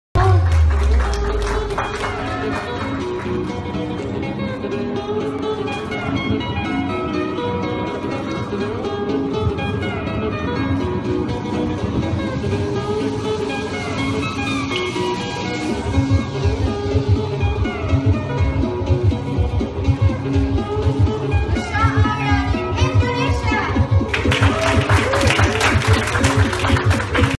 Michelle Victoria Alriani X Yenny Lee Bridal Couture
Thank you @missearthindonesia for sharing my collections on this Miss Earth International Competition 2017 - Press Conference (2 October 2017)